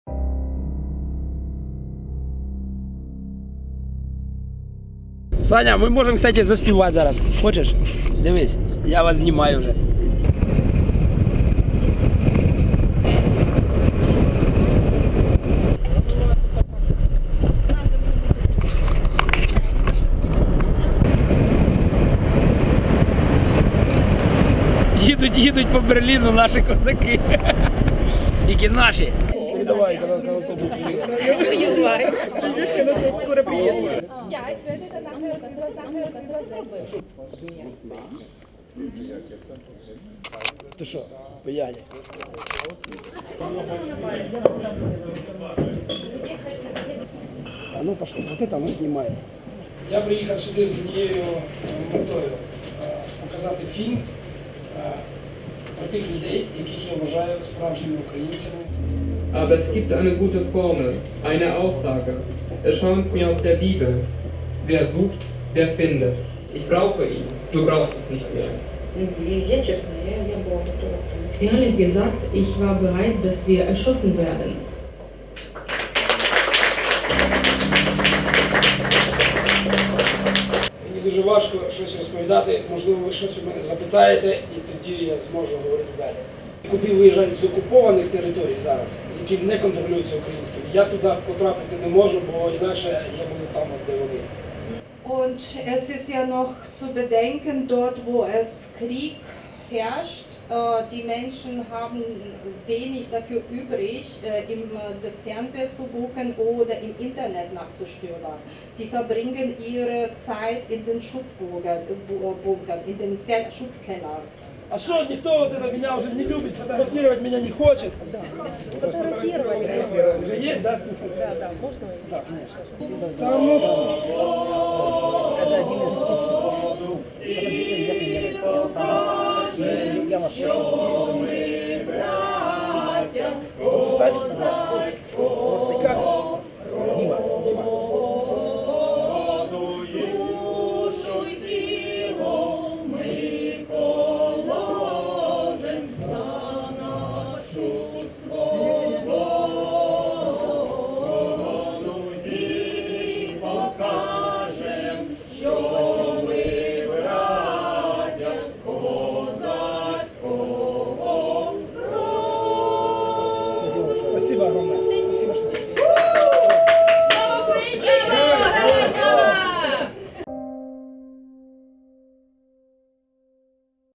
2015.07.05 "Ukropen von Donbas" in Berlin
Die Uraufführung des Dokumentarfilms "Ukropen von Donbas" in Berlin. Wie es war.